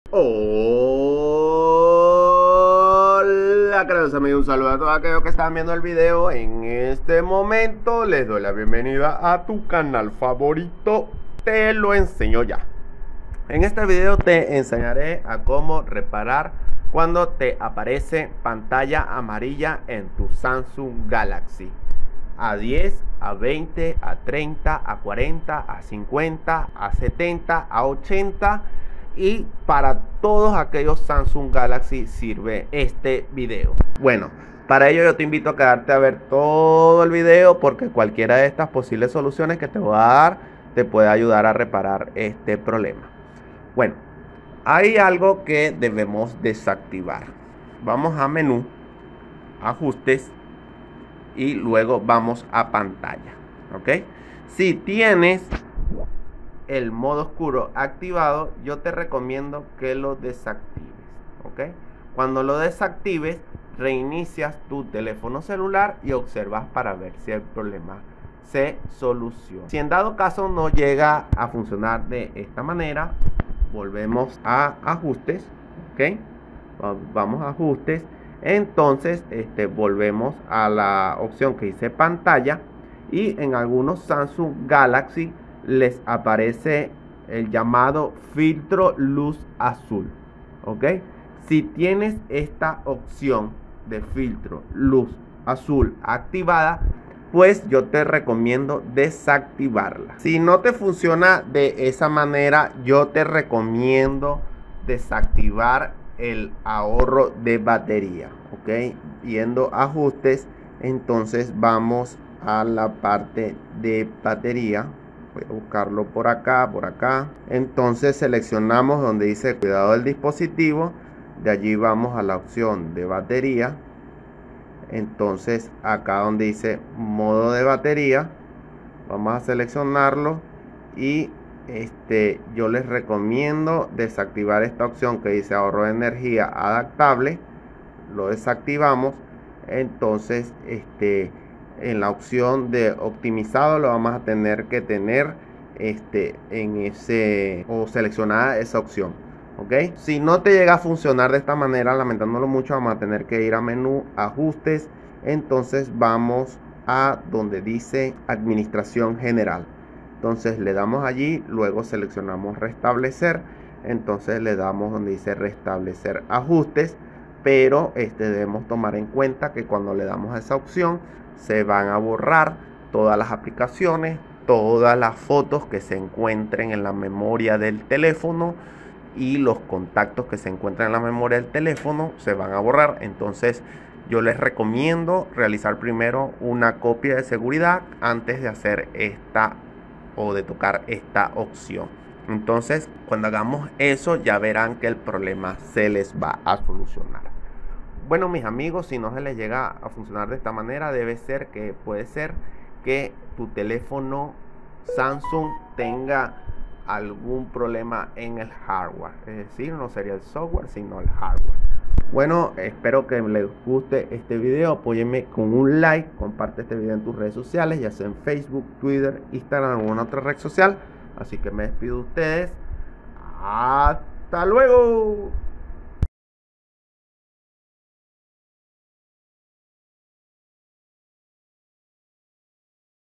Pantalla Amarilla en mi Samsung Galaxy I 3 Soluciones 2025
En este vídeo te ayudare a Como reparar Pantalla Amarilla en mi Samsung Galaxy de una manera rápida y sencilla por medio de 3 soluciones que cualquier persona puede realizar
👇👇👇👇👇👇👇👇👇👇👇👇👇Videos Recomendados
💛 Como ACELERAR Celular